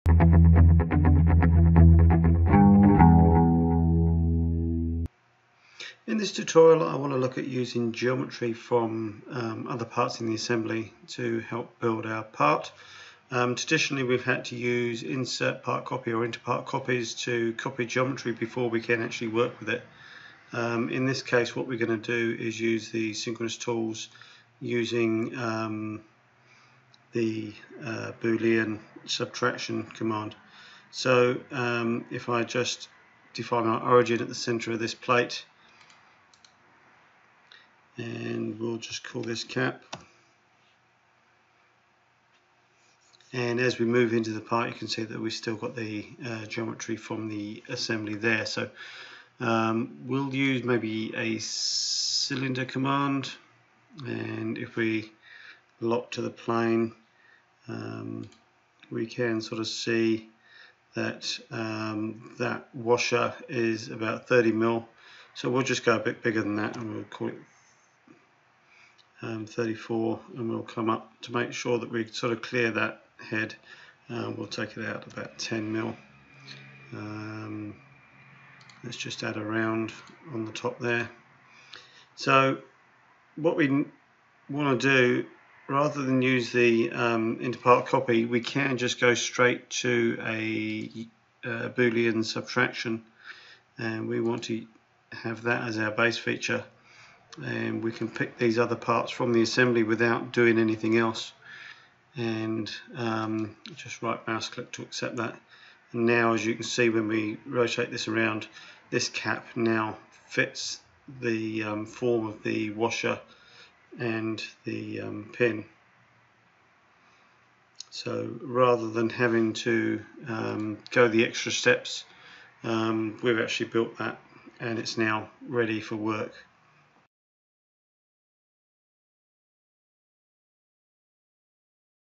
Assembly Subtraction
This tutorial shows how you can use the boolen subtraction command to remove geometry from one part by using the geometry from others, without needing to use part or inter-part copies.